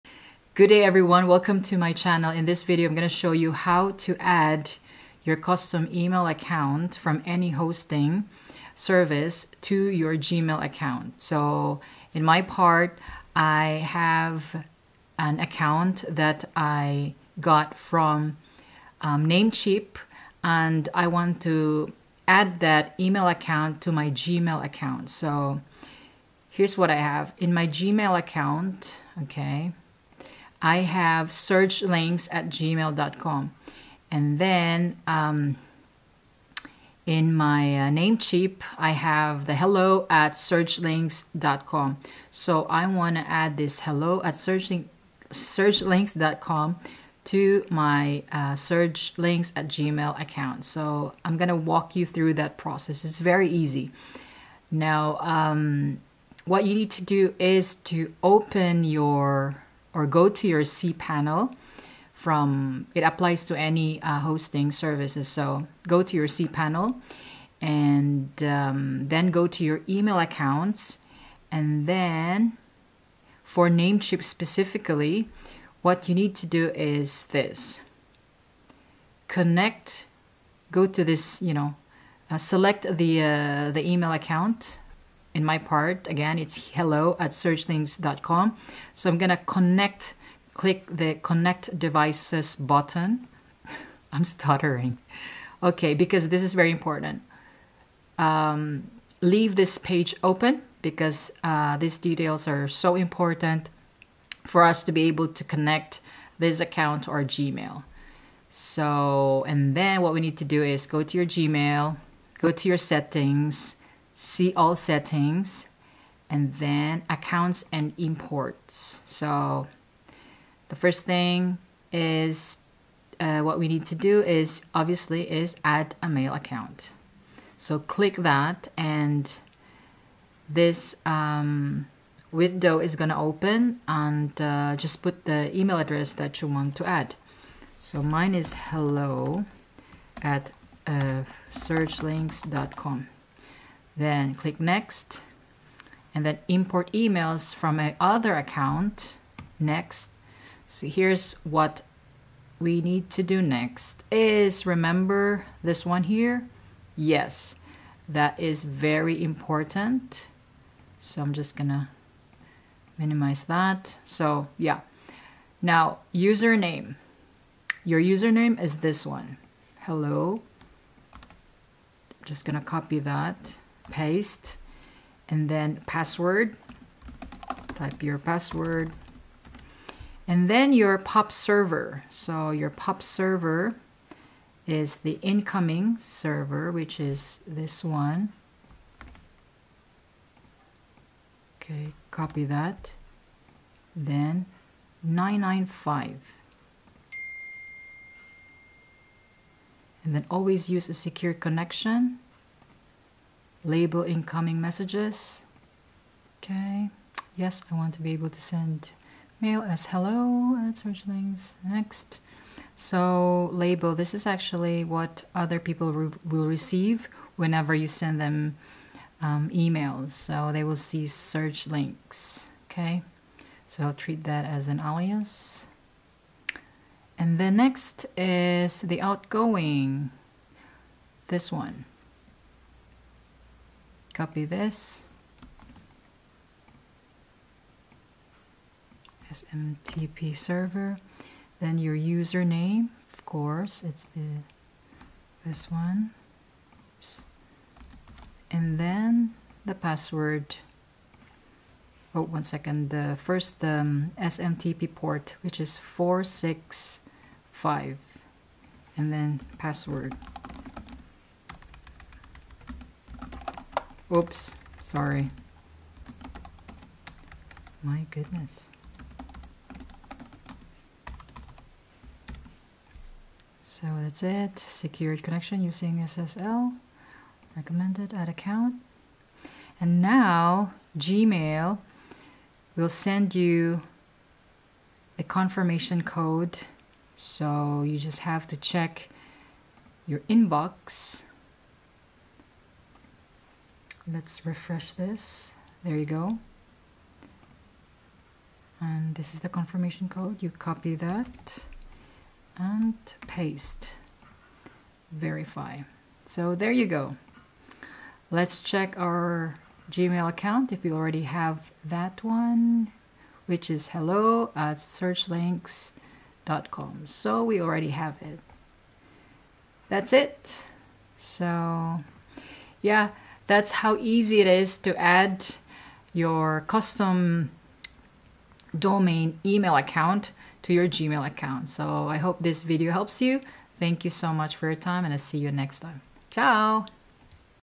How to Connect Namecheap Email to Gmail: Send and Receive Emails Using Your Custom Namecheap Domain
How to connect your custom Namecheap domain mail to your Gmail Account to send and receive emails.
This a very quick and easy step and I'm going to walk you through it!